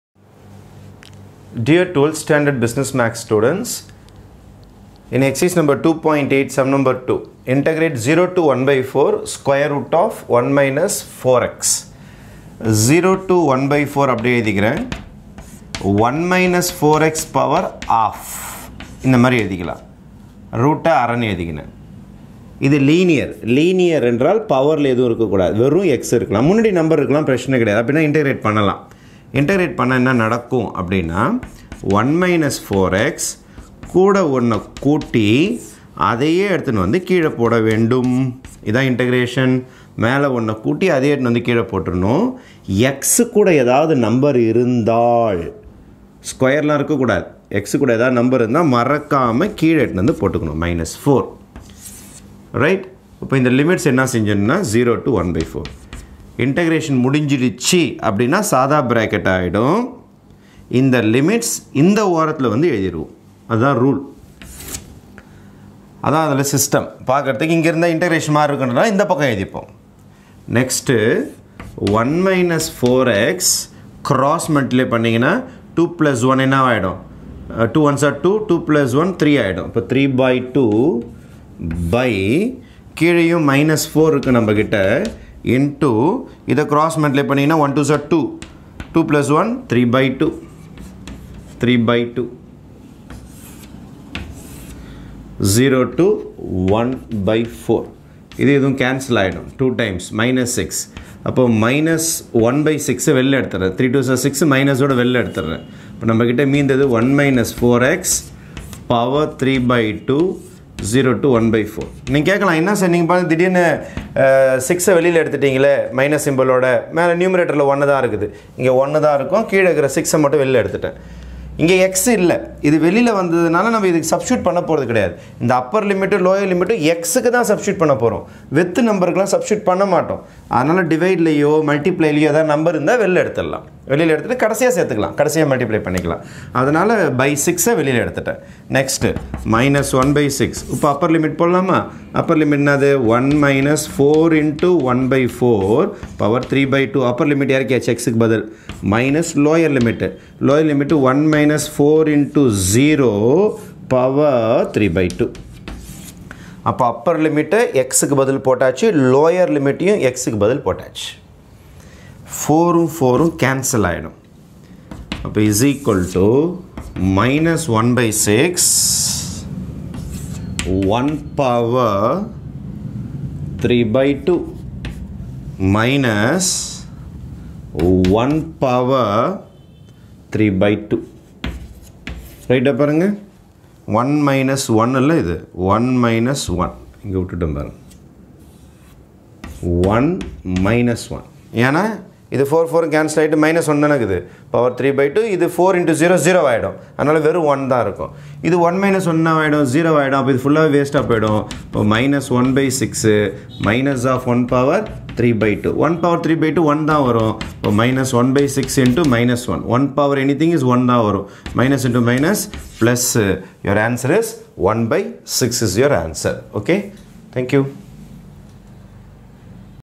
12th Std BM Ex.2.8(2) Using SECOND fundamental theorem Integrate√(1-4x) from 0 to 1/4
BUSINESS MATHS STD.12

CHAPTER 2 INTEGRAL CALCULUS-I